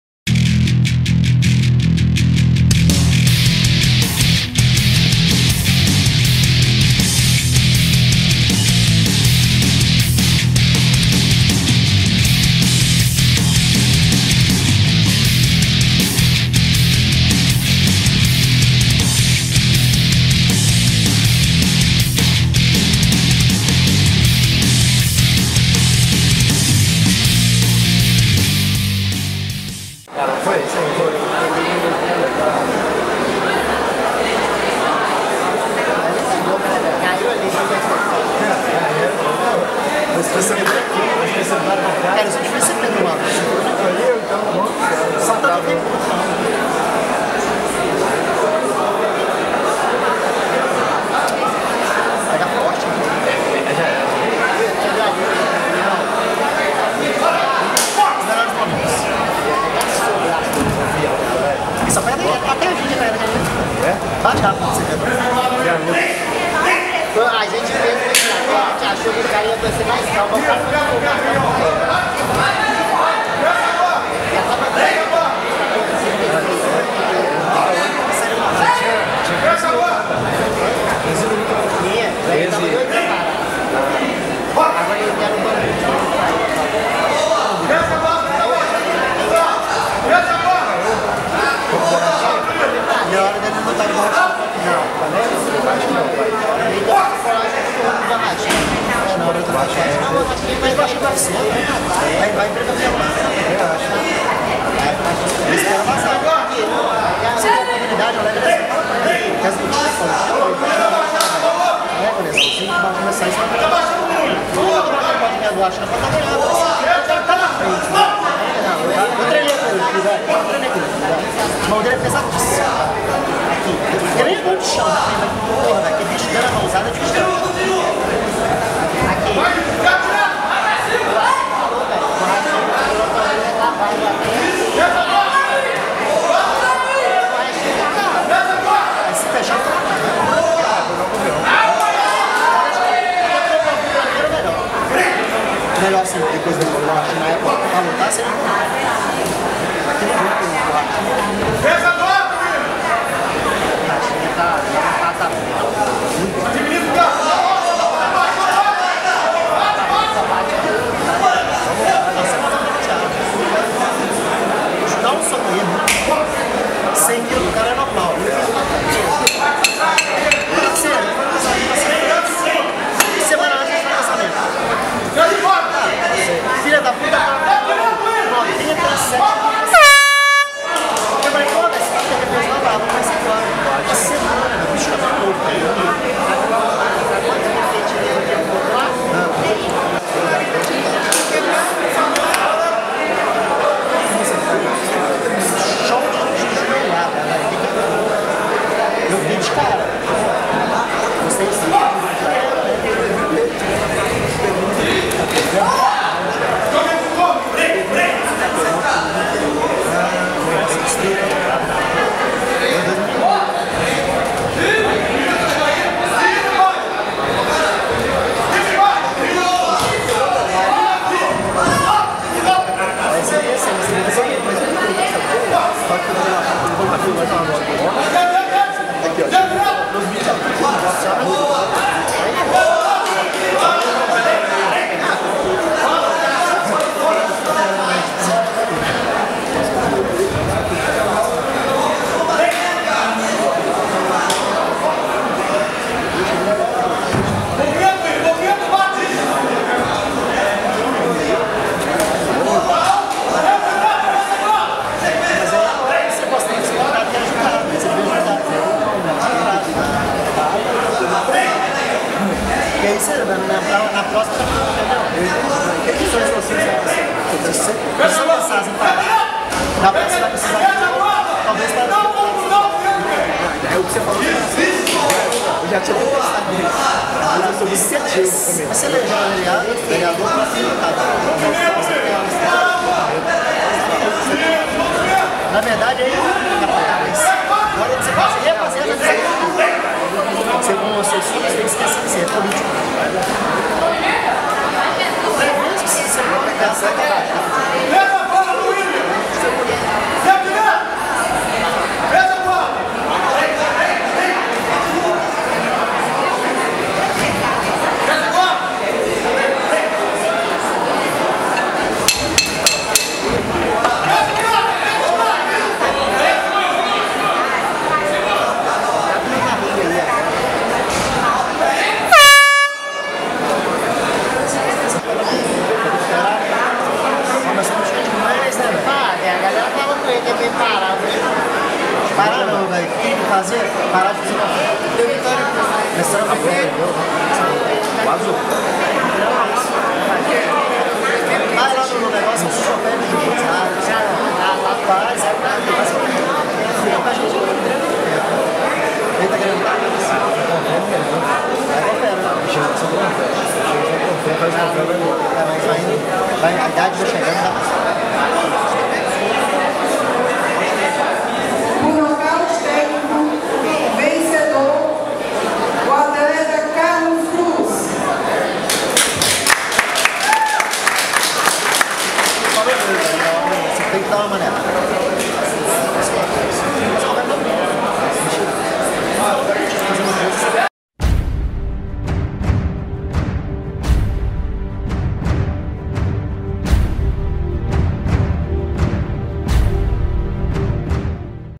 Carlos vs William - BH Sparta 9
Carlos Cruz (Akio Matsuura) veceu Willian Osiris (Sattva Nova Lima) no terceiro round por desistência.